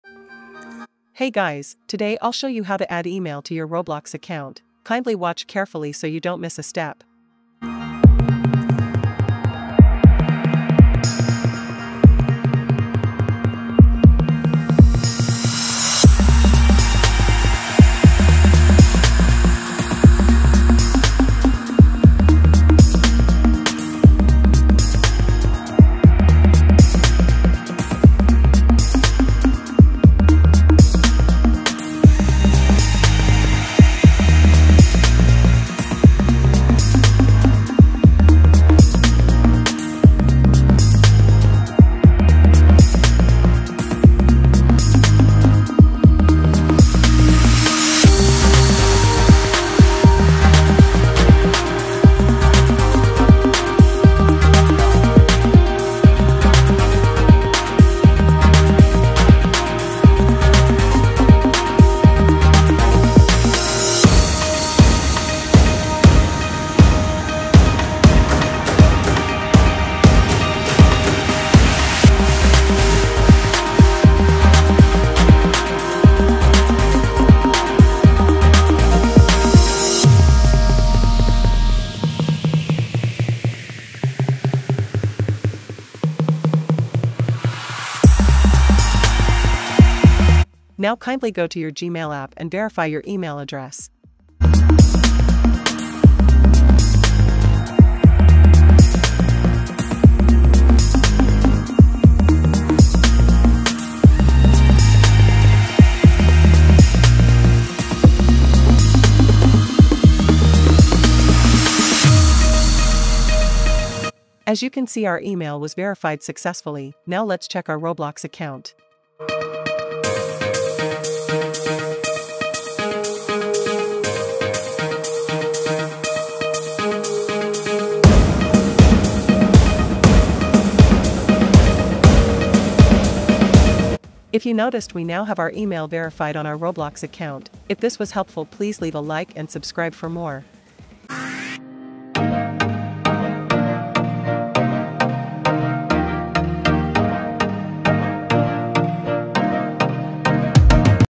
How to ADD EMAIL to your Roblox Account 2025
In this quick tutorial, I'll guide you through the step-by-step process of adding and verifying your email on Roblox in 2 minutes.

Follow Along:

1. Log in to your Roblox account
2. Go to Settings then select Account Info